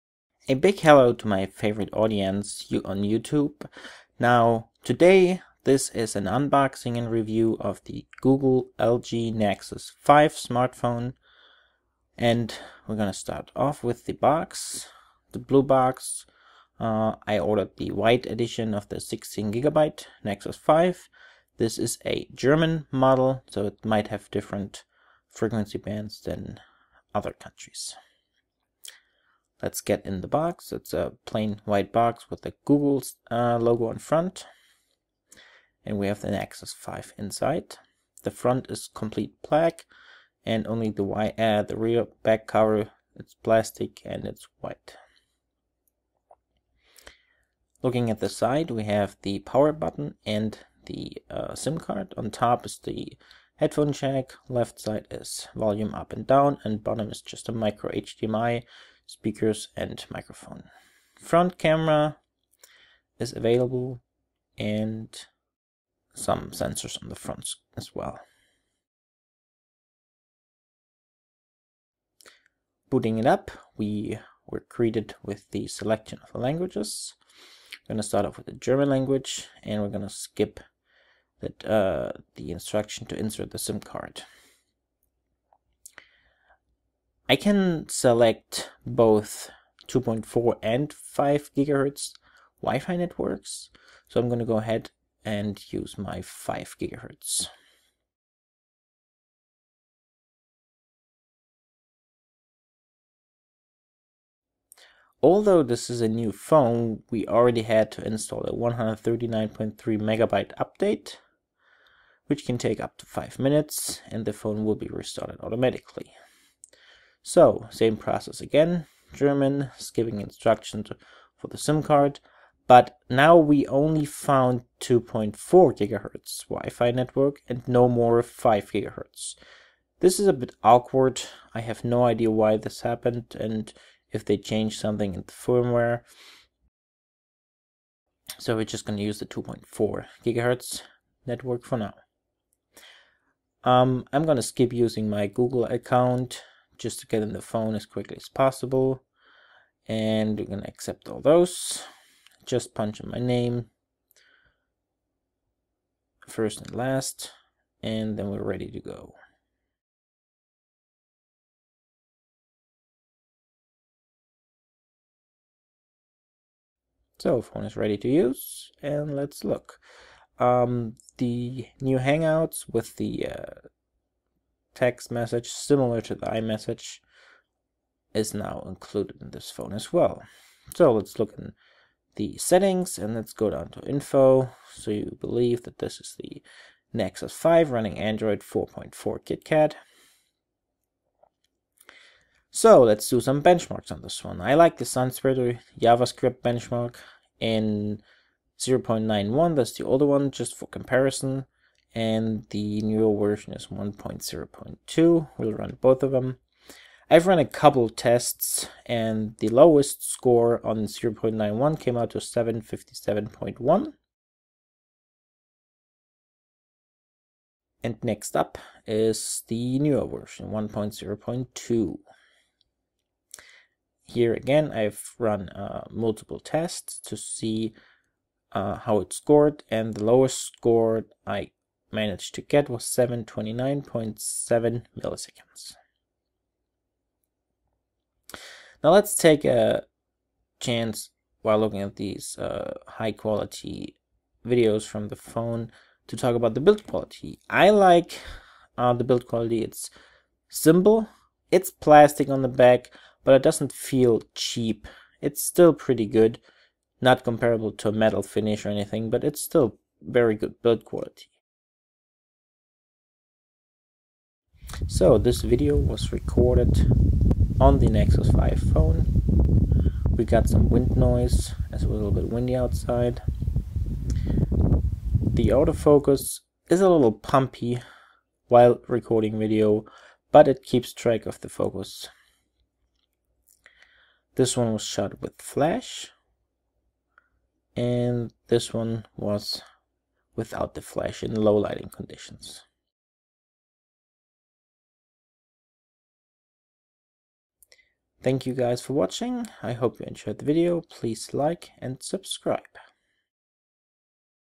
Nexus 5 by Google (LG) 4.95" smartphone unboxing and review FullHD 1080p incl. benchmarks
Welcome to my latest video on YouTube.

I'd like to show you the newest addition to Google's Nexus line-up and introduce you to the Nexus 5 smartphone. It's a 4.95" FullHD Android-based phone running the latest Android 4.4 KitKat. It's powered by a Qualcomm Snapdragon 800 processor and an Adreno 330 GPU. This little powerhouse features all the latest features, such as LTE, NFC, Dualband-WiFi 802.11ac, Bluetooth 4.0, wireless charging, Gorilla Glass 3 and even a barometric sensor. The 2300mAh battery will last you up to 17hrs of talk-time, 300hrs standby-time and up to 8.5hrs WiFi-time. External display support is provided through SlimPort-adapters such as MicroUSB to HDMI. We have a 1.3MP front-facing camera and an 8.0MP rear-facing camera with optical image stabilization. It only weighs 130g and measures 69.17mm x 137.84mm x 8.59mm.

The (DE) model (LG-D821) supports the following bands:
GSM 850/900/1800/1900
WCDMA 800/850/900/AWS/1900/2100 (1/2/4/5/6/8)
LTE 800/850/900/1800/2100/2600 (1/3/5/7/8/20)

I've run a few benchmarks and it proves to be a very powerful phone that can easily compete with other smartphones that are up to twice as expensive as this Nexus 5.

SunSpider JavaScript Benchmark 0.9.1 = lowest time 757.1ms
SunSpider JavaScript Benchmark 1.0.2 = lowest time 729.7ms
3DMark Ice Storm UNLIMITED = 18006 points

The available storage space on a 16GB model will be roughly 12.5GB.

I'm looking forward to your feedback on my video, unboxing and review.